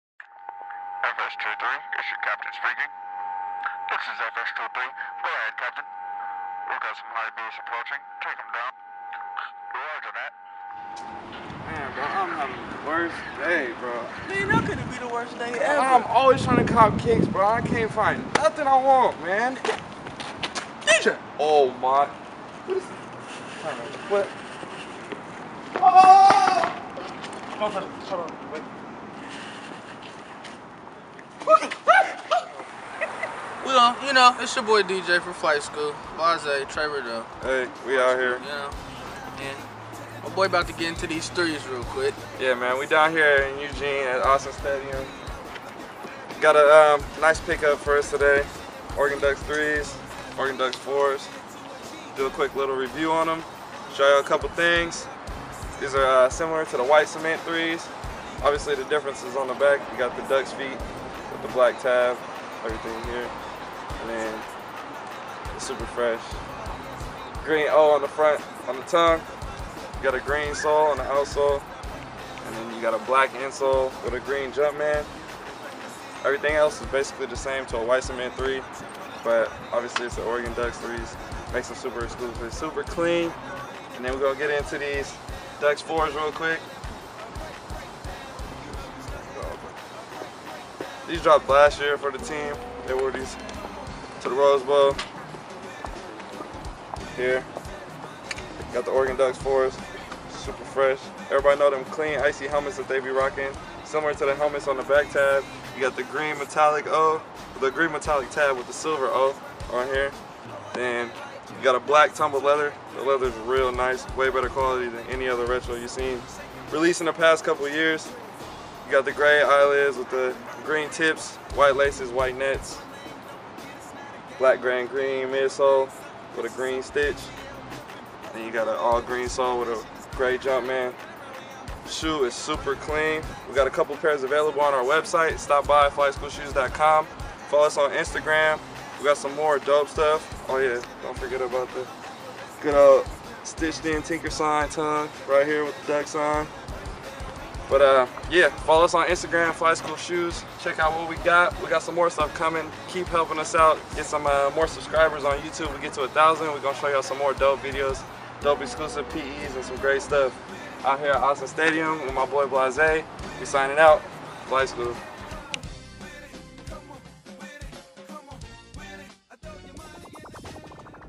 Air Jordan 'Oregon Ducks' 3's & 4's Review at Autzen Football Stadium
We had a chance to go down to the University of Oregon and make our epic review video in front of the Autzen Football Stadium.

You can see more pictures of the Air Jordan 'Oregon Ducks' 3's and 4's on our blog at FLTSKL.blogspot.com